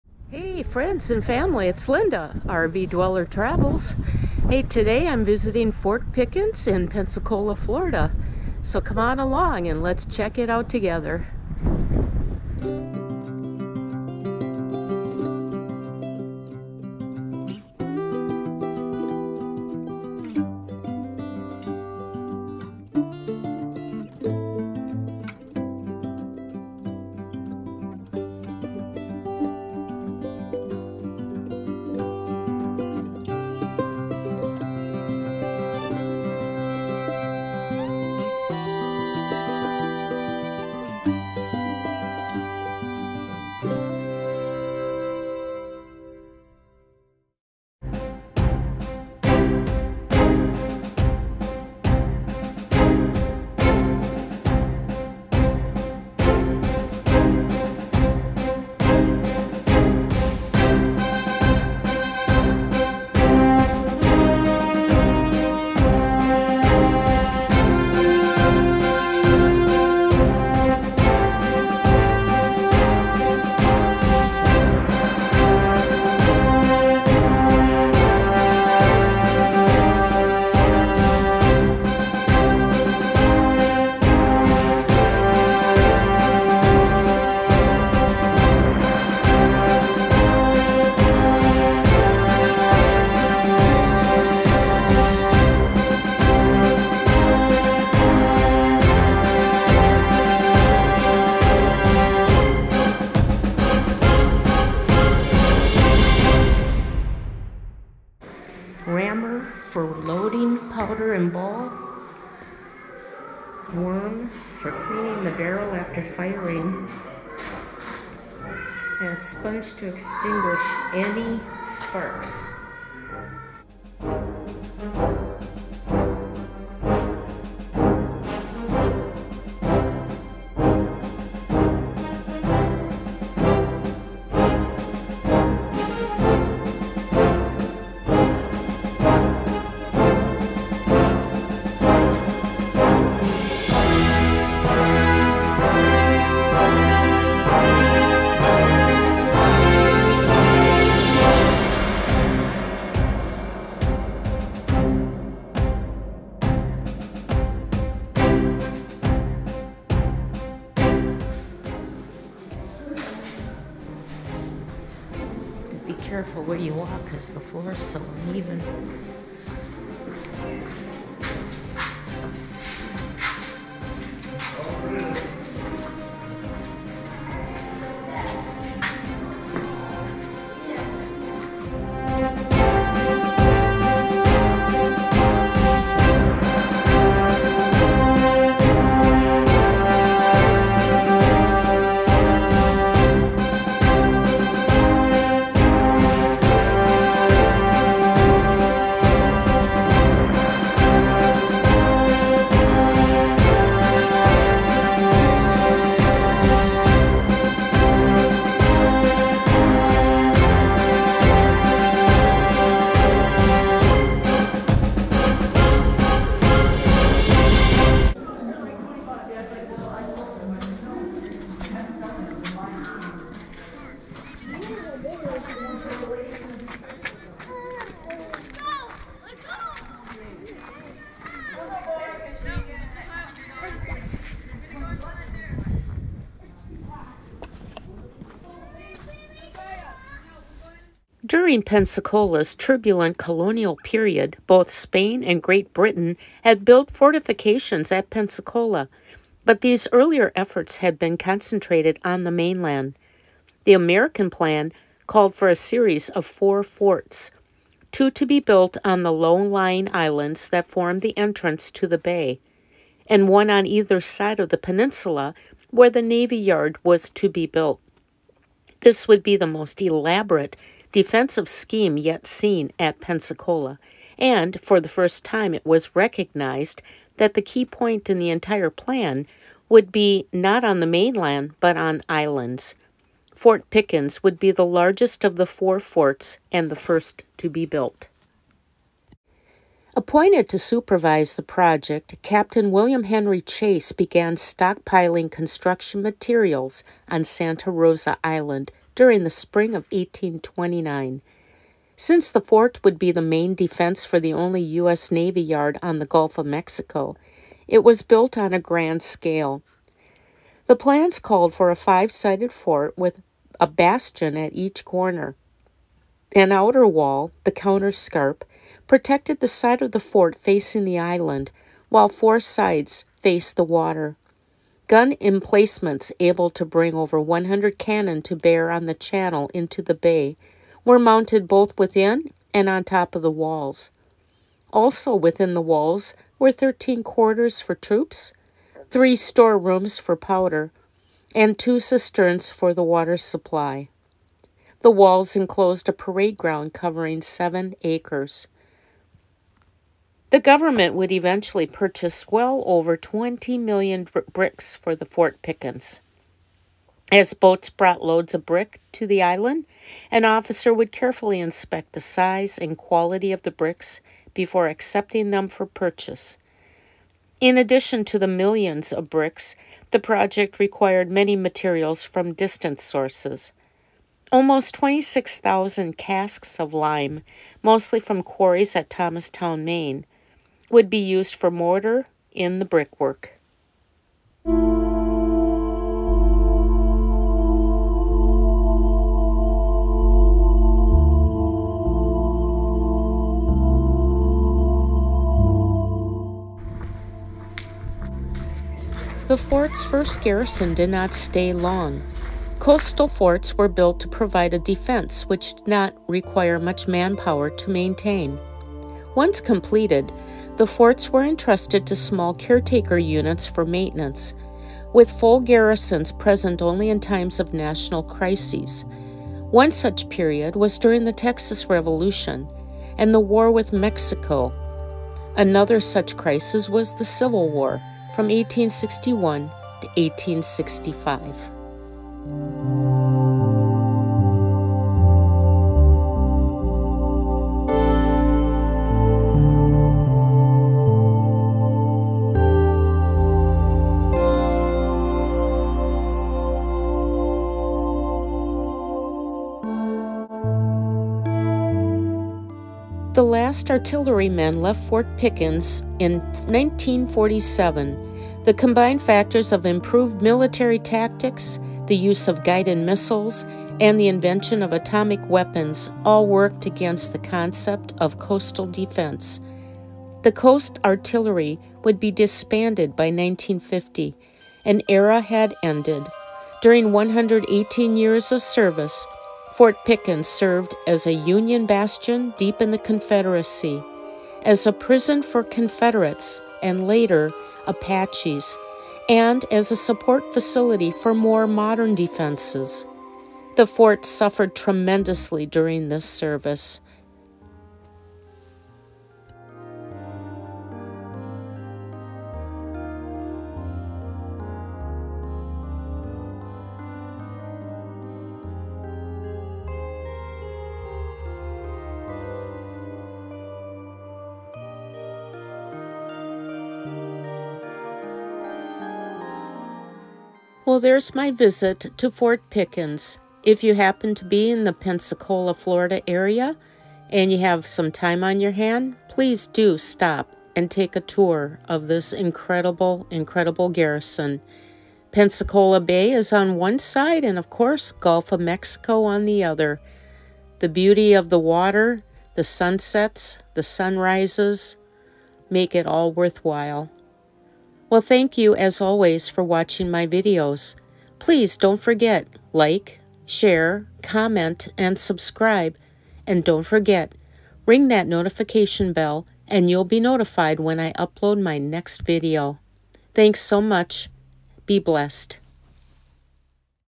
Visit to Fort Pickens
A self-guided walk through historic Fort Pickens in Pensacola, Florida.